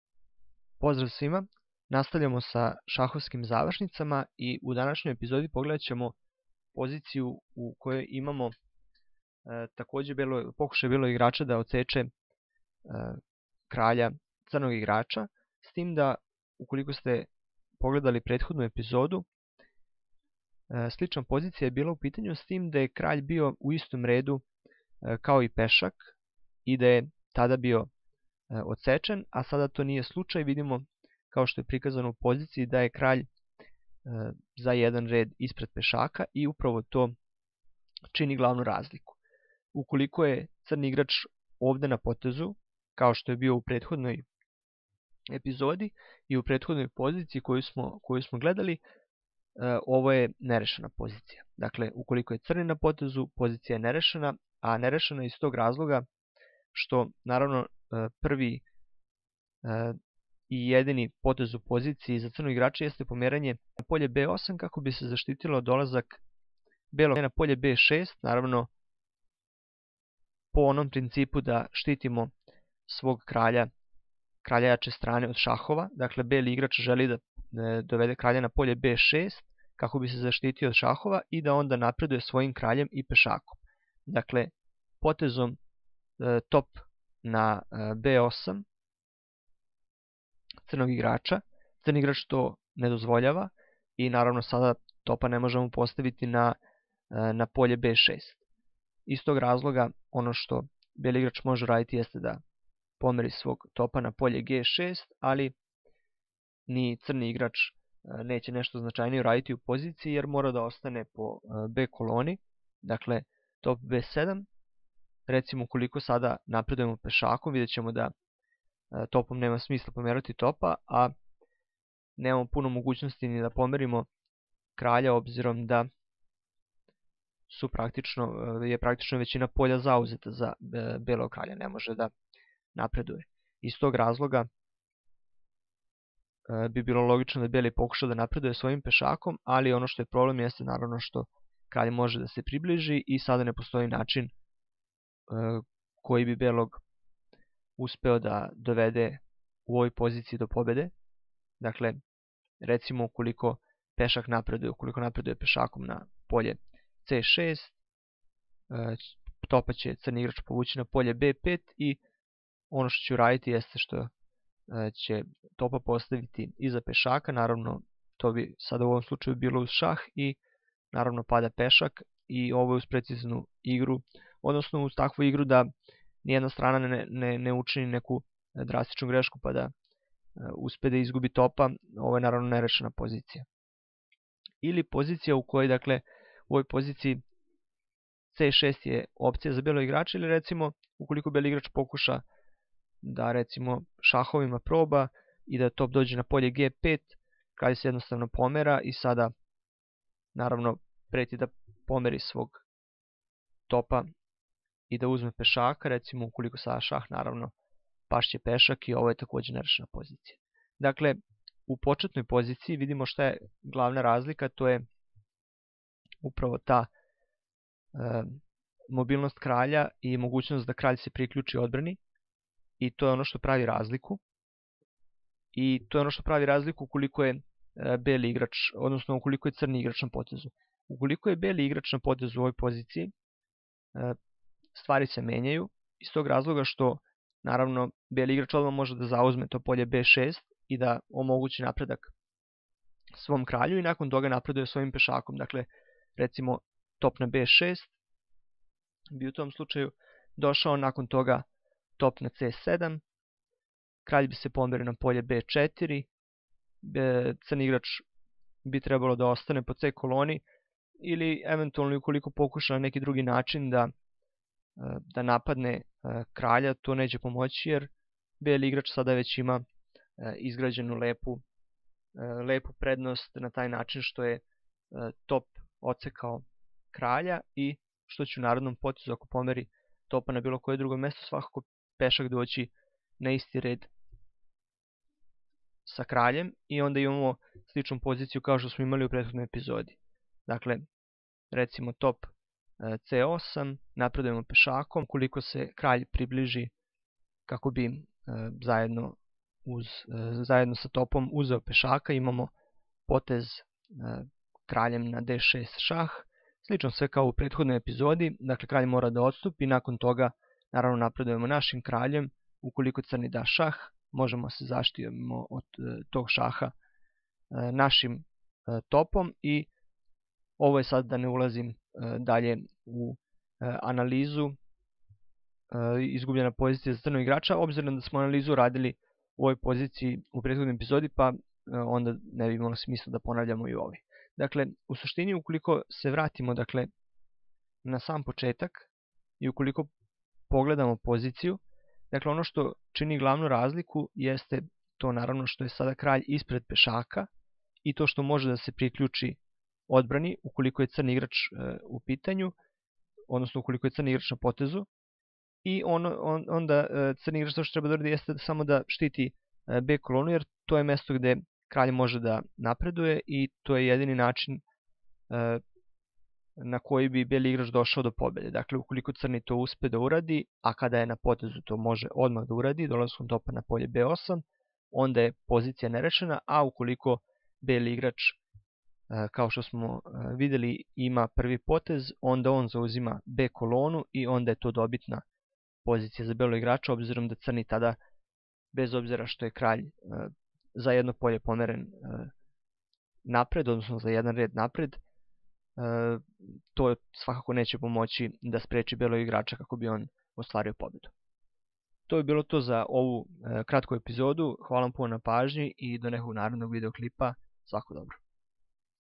100 Zavrsnica koje morate znati - 57. epizoda
Unapredite svoje znanje iz oblasti zavrsnica na pazljivo odabranim primerima sa velikom prakticnom vrednoscu.